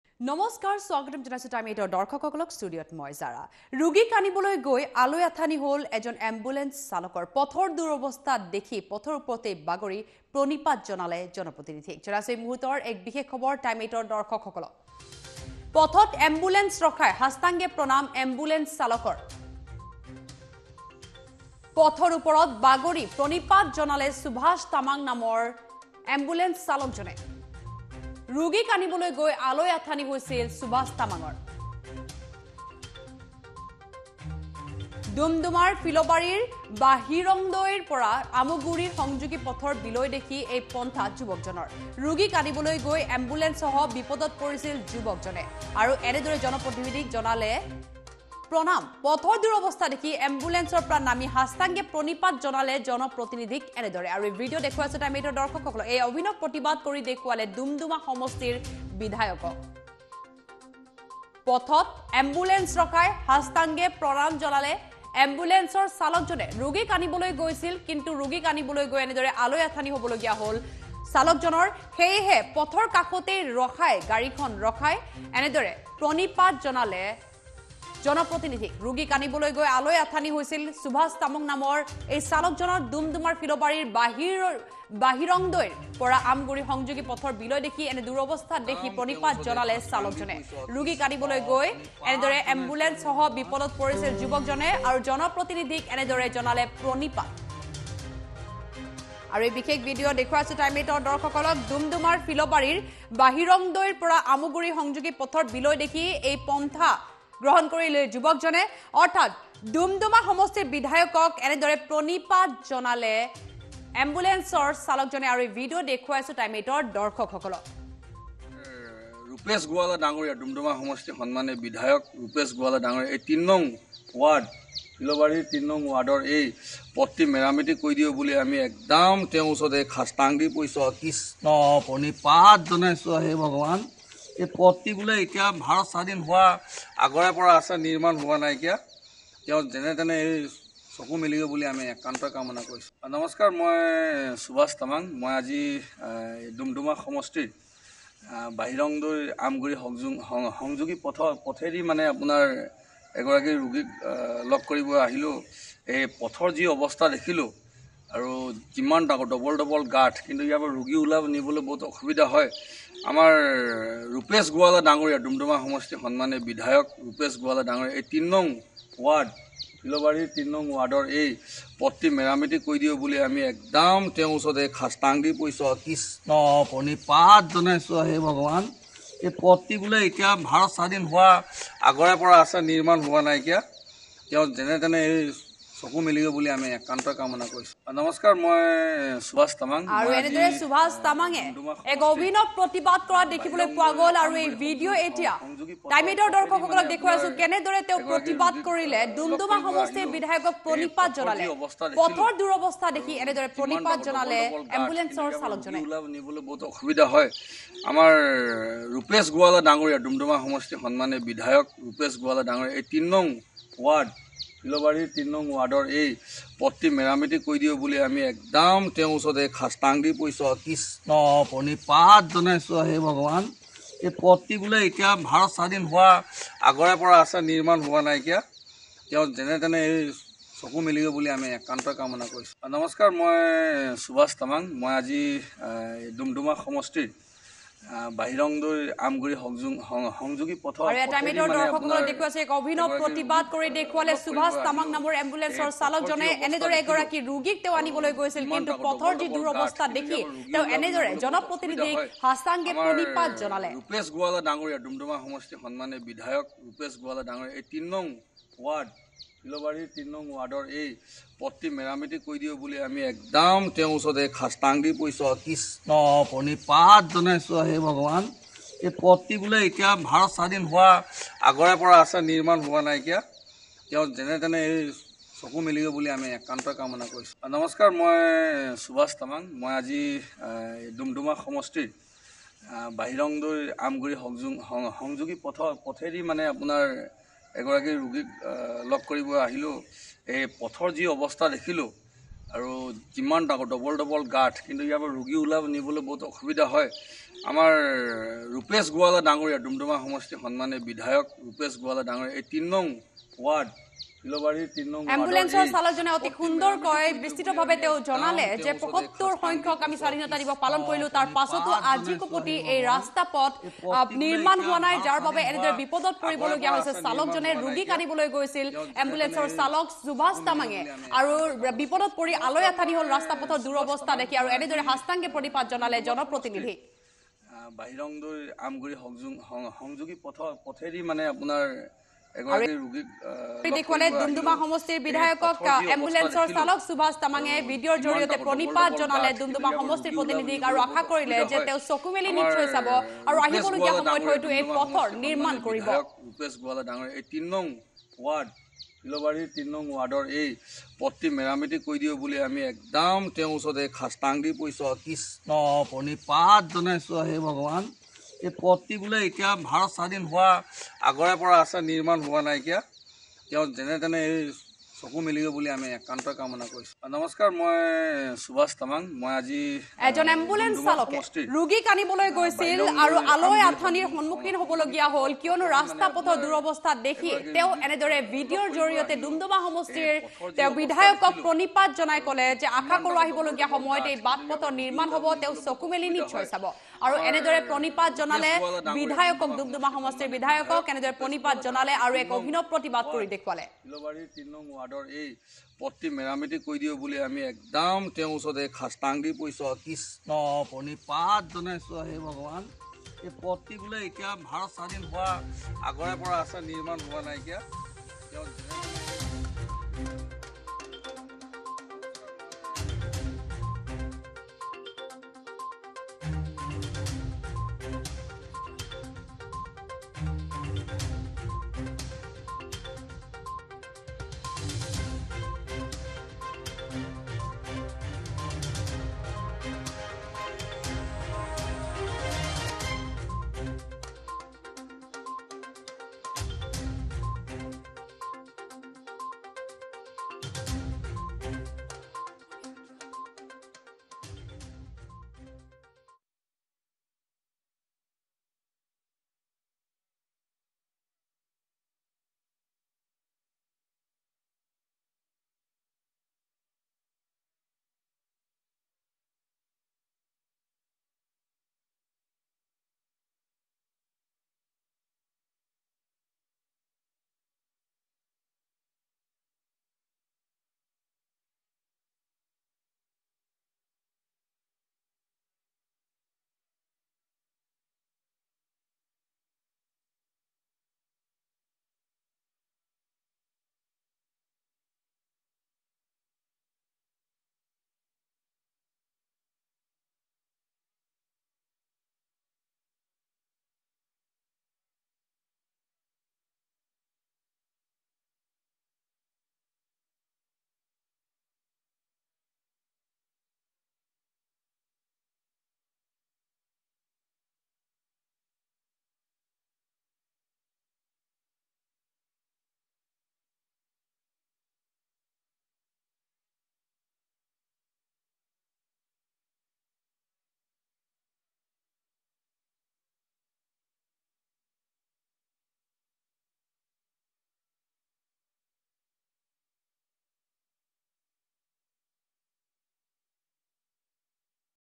পথত এম্বুলেন্স ৰখাই সাষ্টাংগে প্ৰণাম এম্বুলেন্স চালকৰ ।
পথত এম্বুলেন্স ৰখাই সাষ্টাংগে প্ৰণাম এম্বুলেন্স চালকৰ । ডুমডুমাত ৰোগীক আনিবলৈ গৈ আলৈ আথানি এম্বুলেন্স চালক সুভাষ তামাঙৰ